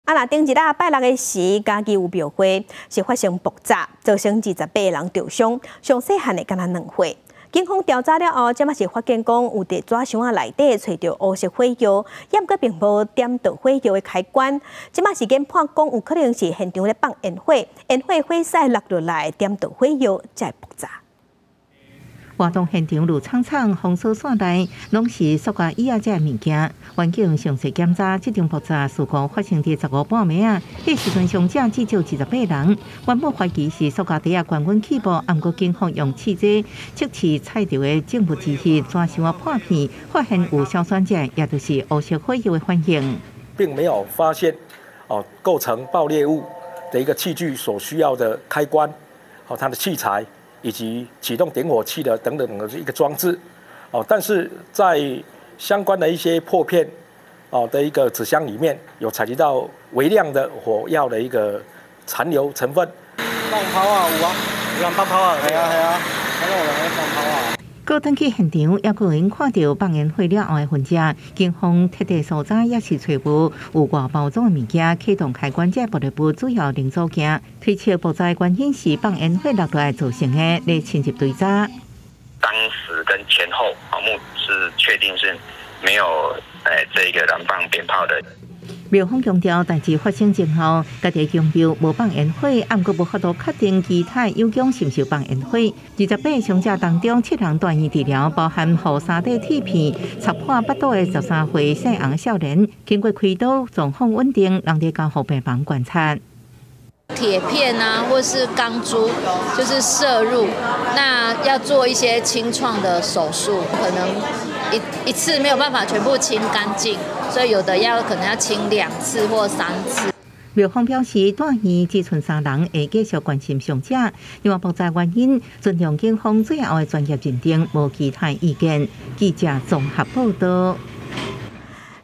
嘉義廟會爆炸害28人傷 疑似是煙火火屎引起 | 公視台語台 | 20230718
拜六的時嘉義廟會活動發生爆炸、造成28人受傷，上細漢只有2歲。警方調查佇紙箱仔內面、揣著黑色火藥，毋過無點火開關、有可能是煙火的火屎、落落來點著火藥，造成爆炸

四節新聞 ：　逐工（每天）09:00 / 12:00 / 15:00（拜一至拜五） / 19:00